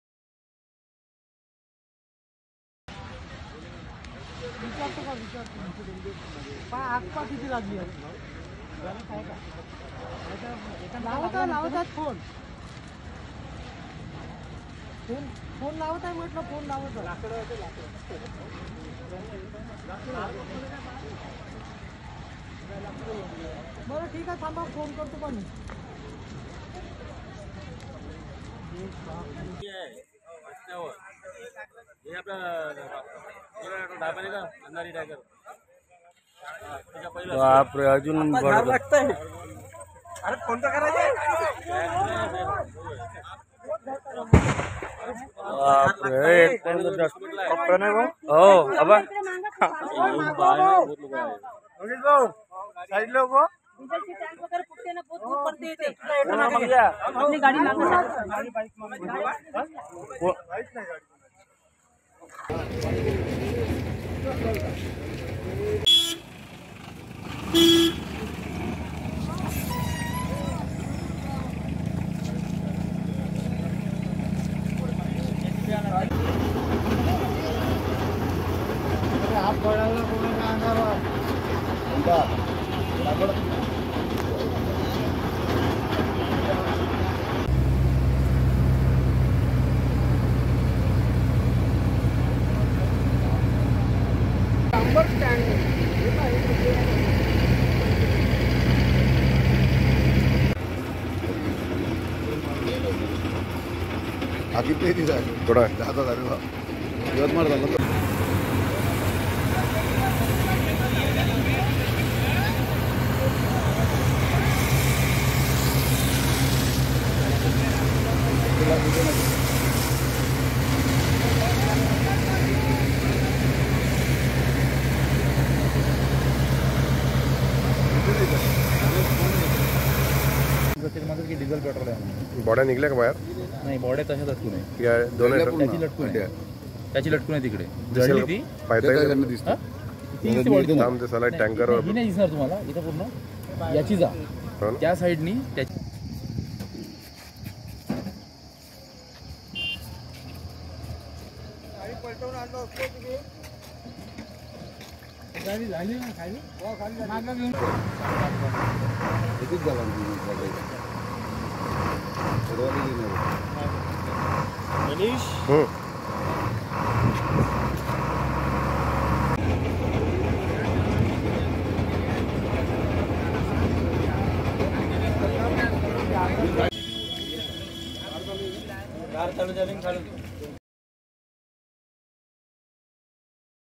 Truck Accident Maharashtra: Truck और Diesel Tanker की टक्कर से भीषण हादसा, 9 Died ।Quint Hindi
Truck Accident Maharashtra: महाराष्ट्र के चंद्रपुर जिले में भीषण सड़क हादसा हुआ है. डीजल टैंकर और ट्रक की जोरदार भिड़ंत के बाद वाहनों में लग गई. आग में जलने के कारण 9 लोगों की मौत हो गई है. हादसे में ट्रक चालक की भी जान चली गई है.बताया जा रहा है कि ये हादसा चंद्रपुर जिले के एक इलाके में हुआ है. हादसे के सूचना मिलते ही पुलिस मौके पर पहुंच गई. पुलिस अधिकारियों ने बताया कि ट्रक में लकड़ियां लदी हुई थी. फिलहाल हादसे की जांच की जा रही है.